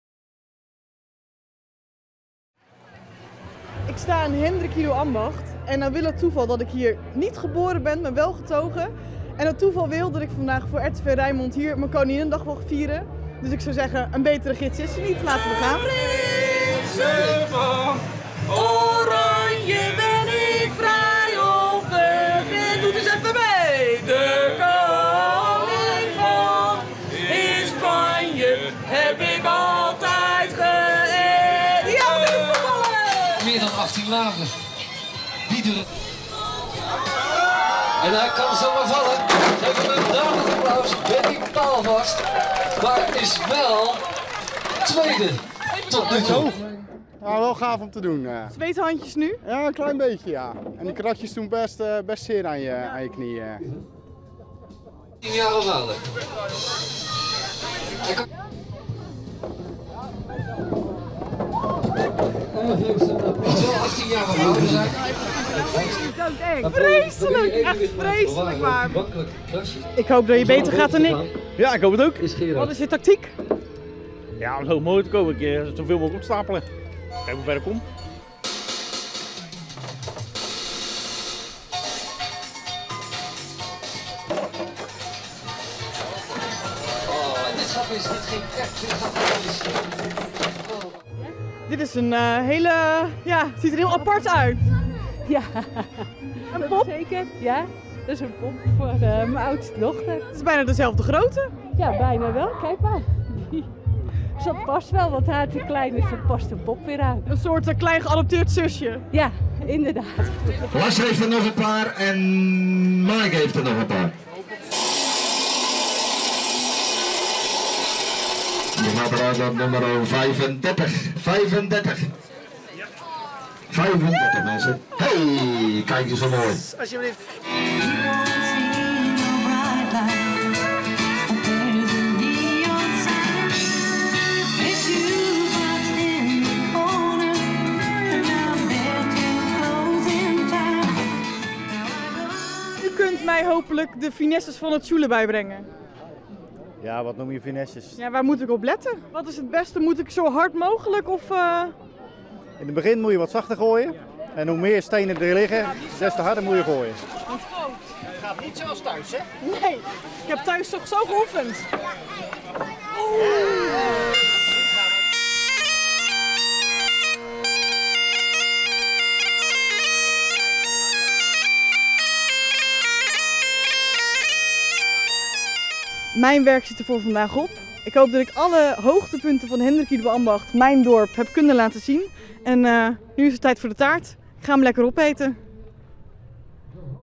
Feest in Hendrik-Ido-Ambacht
Op veel plaatsen in de regio is enthousiast Koninginnedag gevierd. Tot nu toe verloopt alles in een goede sfeer zonder incidenten. Vrijmarkten, kermissen, muziekfeesten en spelletjes bepalen het beeld.